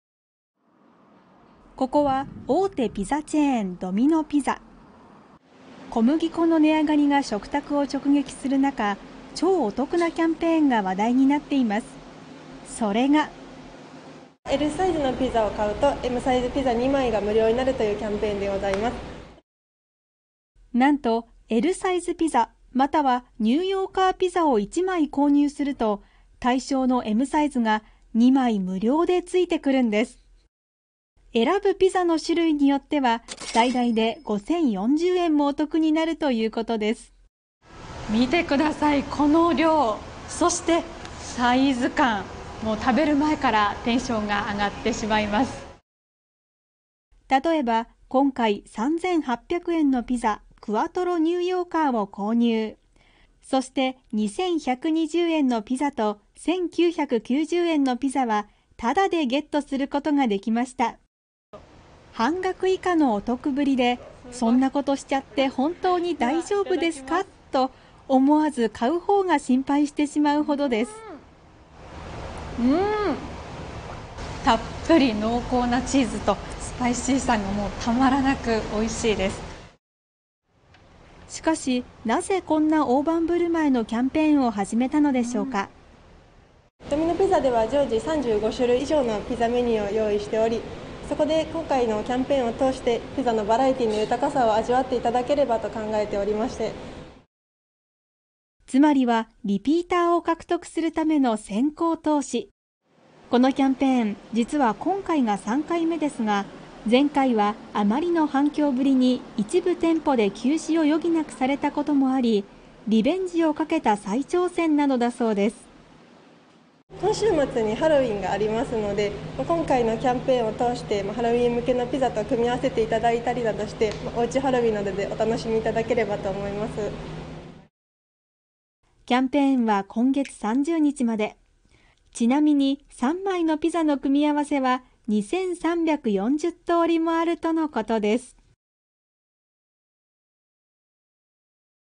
超お得！ピザを1枚買うと2枚タダでついてくる…大盤振る舞いなキャンペーンが話題に
様々なモノが値上がりする中、小麦粉の高騰は特に家計に大きな打撃を与えています。こうした中、ある「超お得」なキャンペーンが話題になっています。1つ買うと、なんと２つタダで付いてくるんです。